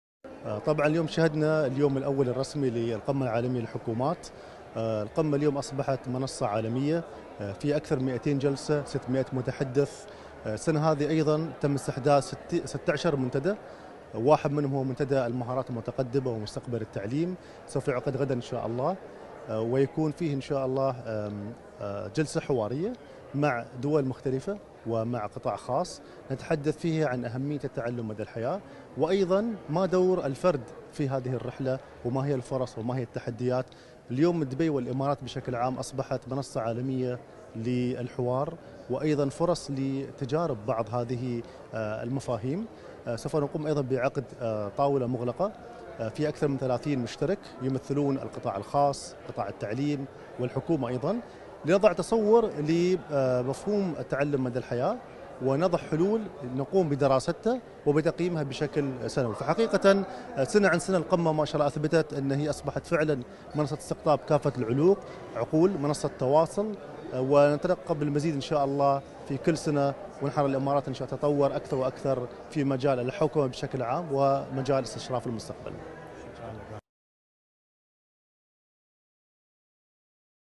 لقاء فرسان الإمارات مع معالي د. أحمد بالهول الفلاسي
لقاء فرسان_الإمارات مع معالي د. أحمد_بالهول_الفلاسي، وزير الدولة لشؤون التعليم العالي والمهارات المتقدمة خلال انطلاق فعاليات القمة_العالمية_للحكومات في دورتها السابعة بـ دبي.